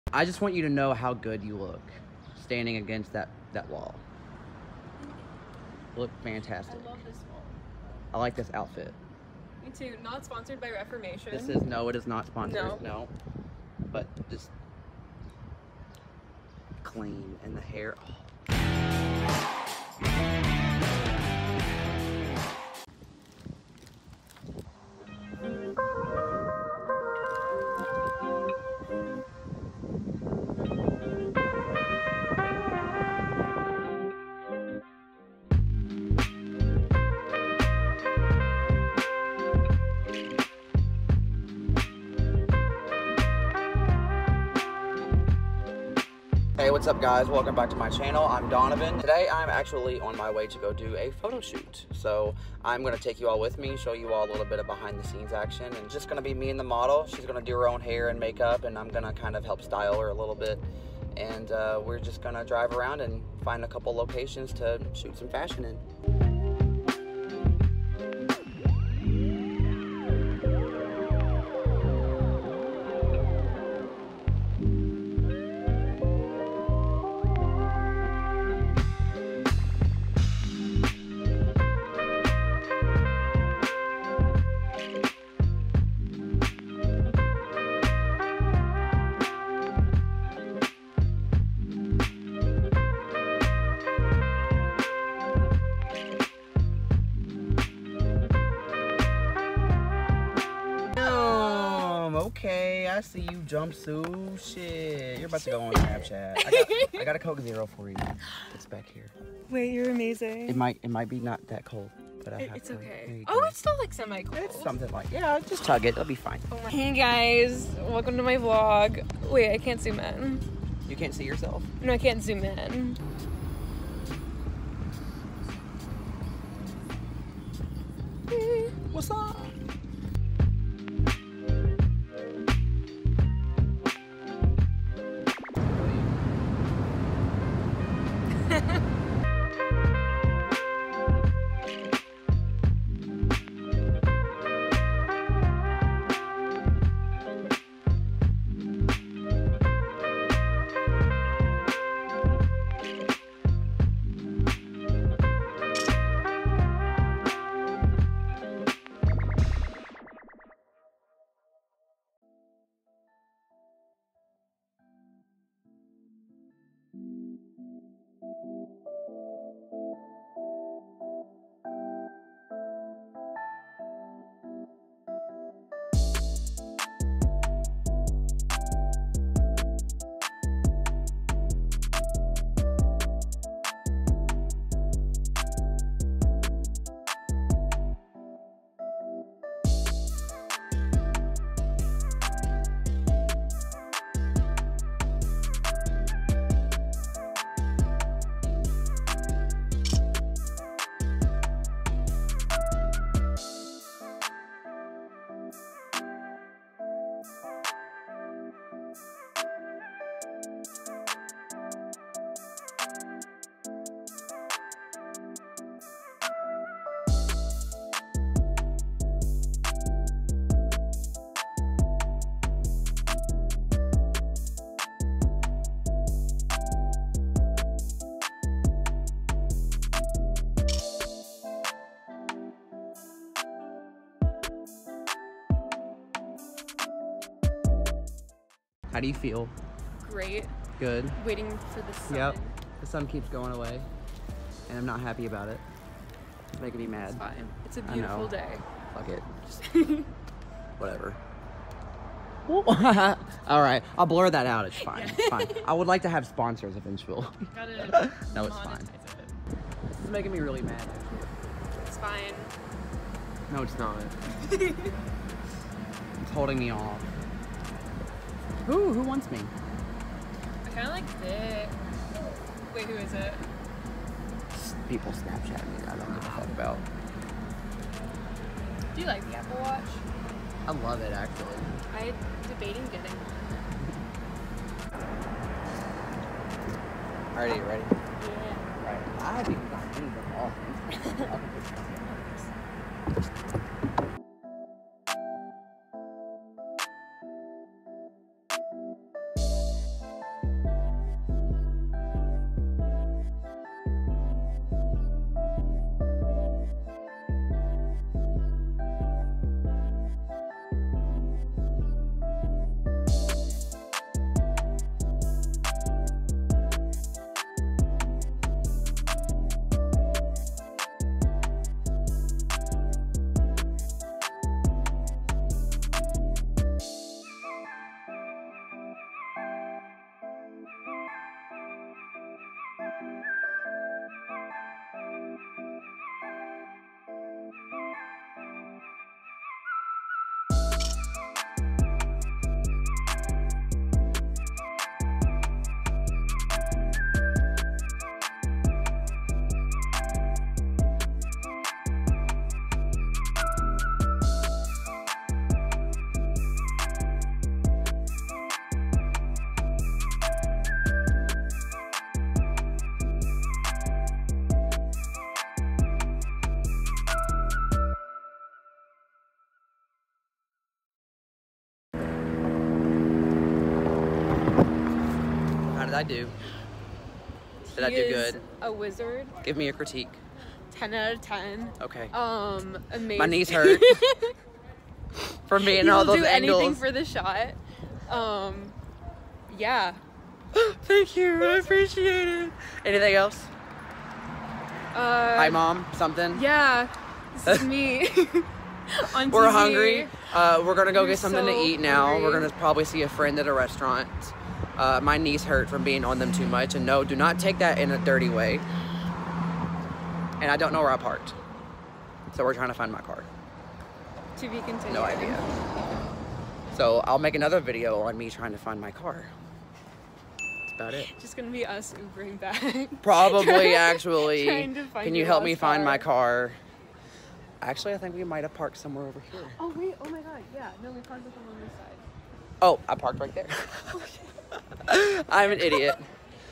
Fashion Photoshoot - Behind the Scenes
Behind the scenes of a golden hour fashion photoshoot with model and photographer Elizabeth Carrdigan.

GEAR USED:
Canon 5D Mark III
50mm 1.2 lens
24-70mm 2.8 lens

Processed in Lightroom, retouched and color graded in Photoshop CC 2019.